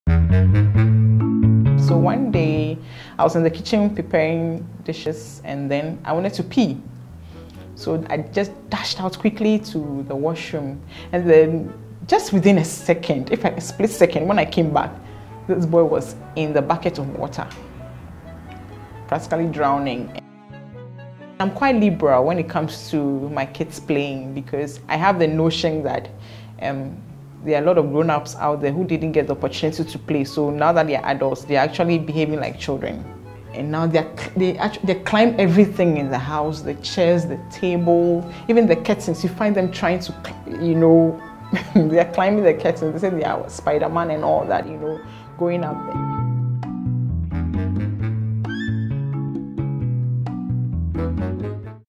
MY SON THINKS HE"S SPIDERMAN _ Eyram on MUMMY TALES _ Trailer
COMING UP NEXT:
Eyram a mother of 3 children, talks about an emergency during her delivery, the transition from being a working-mum to a stay-at-home mum, coping active children and a lot more!

We are a team that's dedicated to telling the Ghanaian stories surrounding pregnancy, childbirth and parenting.